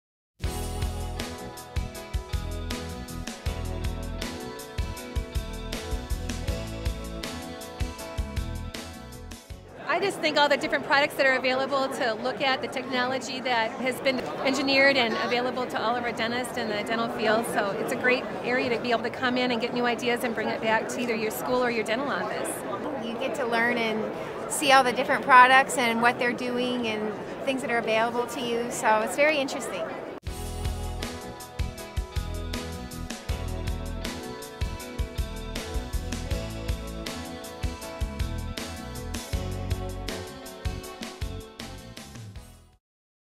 Chicago Dental Society 2011 Midwinter Meeting - Dental Hygienists on the Midwinter Meeting
Comments from attendees during the 2010 Midwinter Meeting. Join us February 24-26, 2011 for the 146th Midwinter Meeting at McCormick Place West Building.